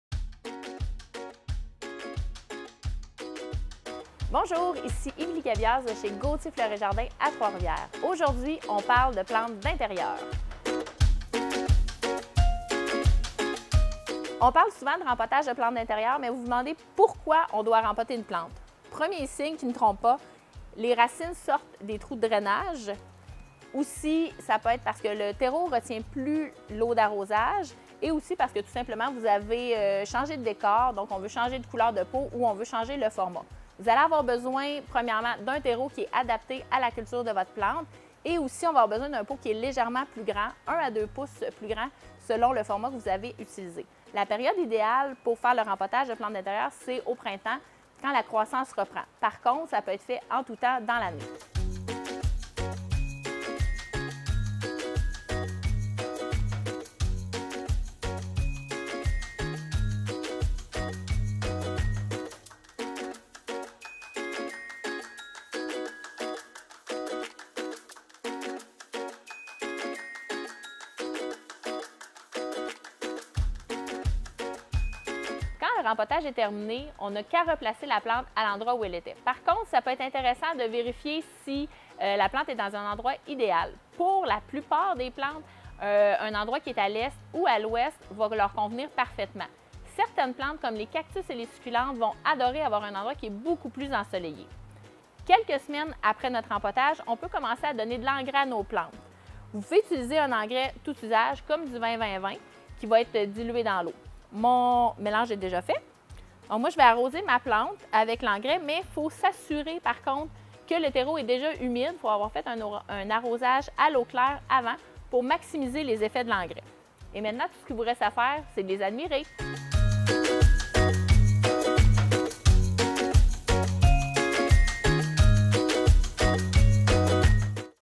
Rempoter les plantes d'intérieur
Rempoter ses plantes d’intérieur… où, quand et pourquoi au juste?

Dans cette vidéo, Émilie Gabias de chez Gauthier Fleurs et Jardins à Trois-Rivières vous fait prendre conscience des signes auxquels il faut porter attention et vous montre comment s’y prendre pour rempoter sans souci, ainsi que les meilleurs produits d’entretien à utiliser.